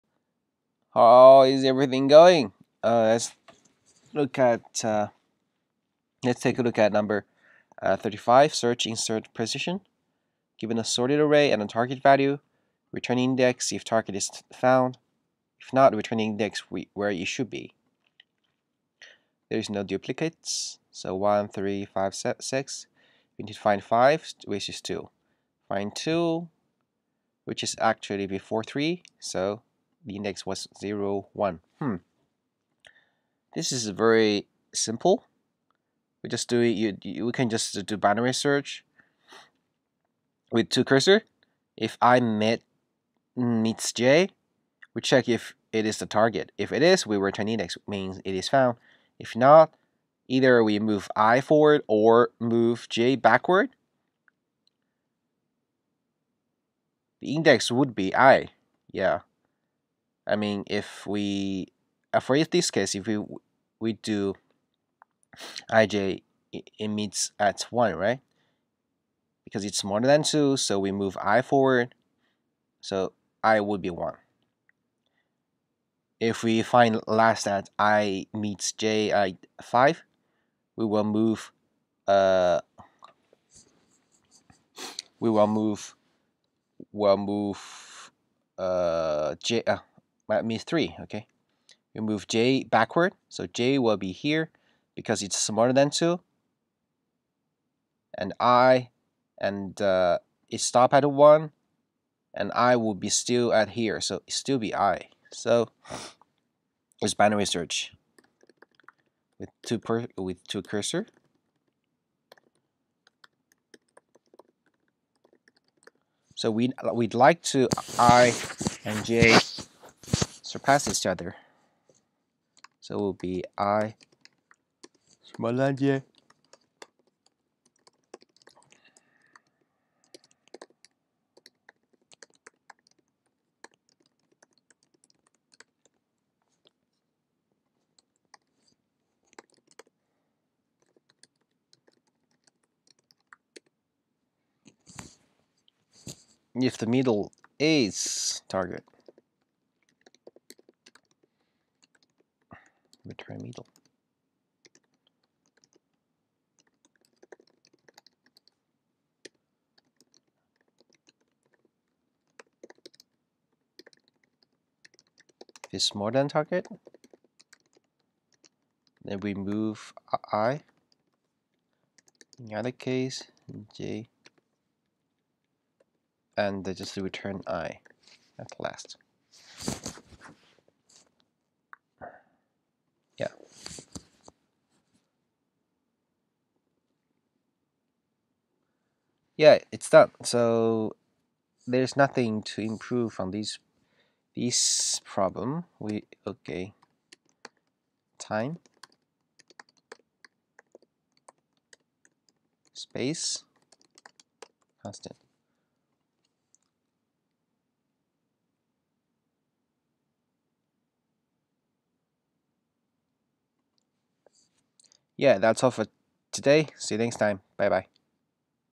leetCode 35 Search Insert Position (Binary Search) | JSer - algorithm & JavaScript
leetCode 35 Search Insert Position (Binary Search)
Pretty easy problem, take care of the ending case.

and currently practicing leetCode online. If you are interested, maybe we can learn together.